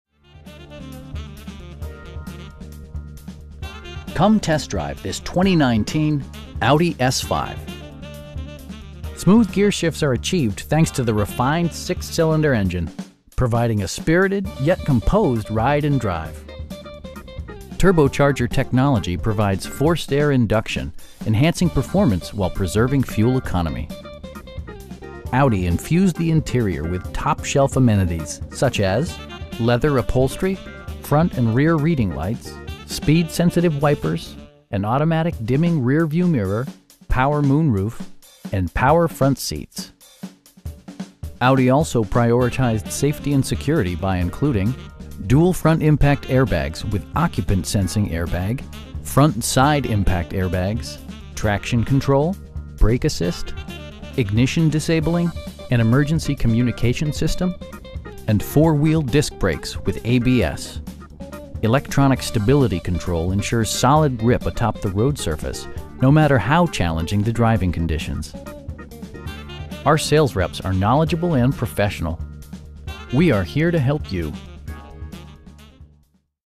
2019 Audi S5 3.0T Premium Plus in Carlsbad, CA 92008
Audi Carlsbad
5425 Paseo Del Norte

Come test drive this 2019 Audi S5. Smooth gearshifts are achieved thanks to the refined 6 cylinder engine, providing a spirited, yet composed ride and drive. Turbocharger technology provides forced air induction, enhancing performance while preserving fuel economy. Audi infused the interior with top shelf amenities, such as: leather upholstery, front and rear reading lights, speed sensitive wipers, an automatic dimming rear-view mirror, power moon roof, an overhead console, and power front seats. Audi also prioritized safety and security by including: dual front impact airbags with occupant sensing airbag, front side impact airbags, traction control, brake assist, ignition disabling, an emergency communication system, and 4 wheel disc brakes with ABS. Electronic stability control ensures solid grip atop the road surface, no matter how challenging the driving conditions. Our sales reps are knowledgeable and professional. They'll work with you to find the right vehicle at a price you can afford. We are here to help you.